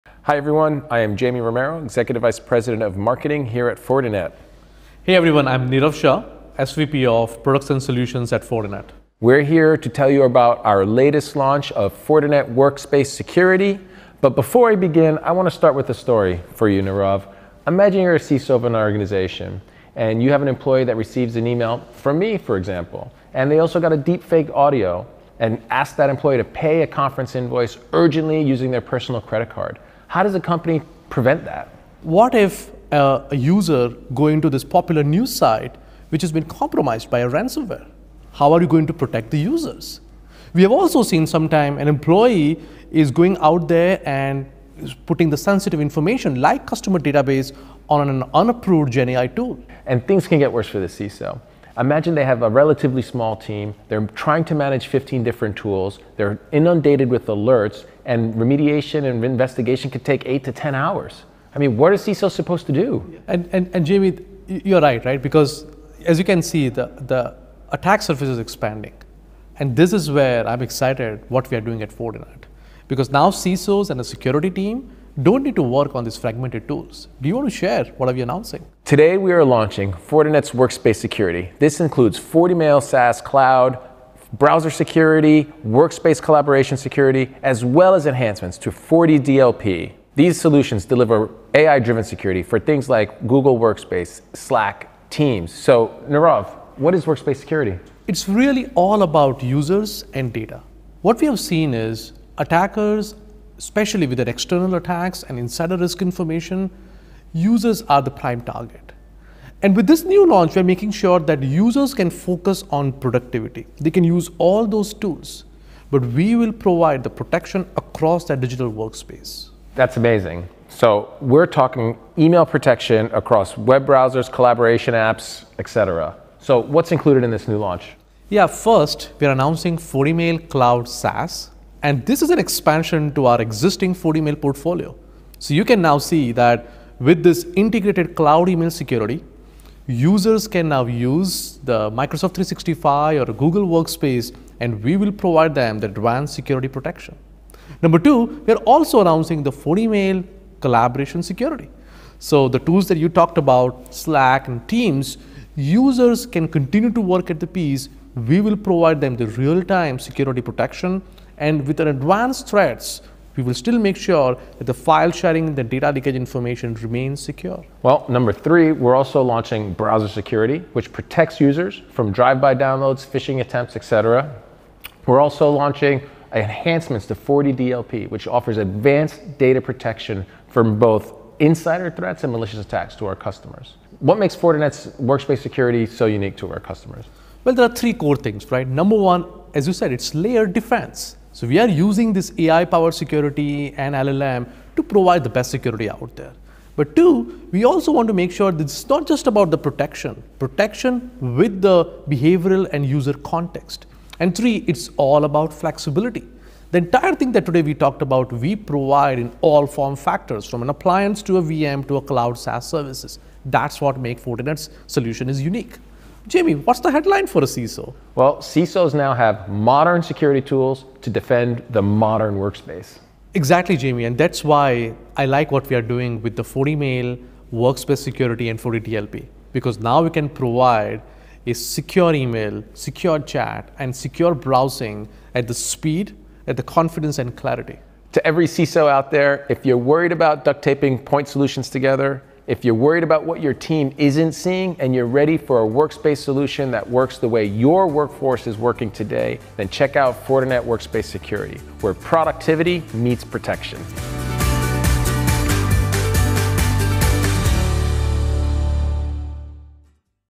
AI-Powered Workspace Security Suite to Protect the Modern Enterprise | Fortinet Workspace Security
In this video, Jamie Romero and Nirav Shah from Fortinet explore how CISOs can tackle today’s top cybersecurity challenges—including the rise of deep fake audio scams and alert overload.

Learn how Fortinet’s new workspace security solutions offer advanced protection for:
🔒 SaaS applications
🔒 Collaboration tools
🔒 Browser environments

By integrating security directly into everyday tools, these solutions help reduce remediation time, boost user productivity, and defend against both external and internal threats.

Stay ahead of evolving cyber risks with a safer, more secure digital workspace.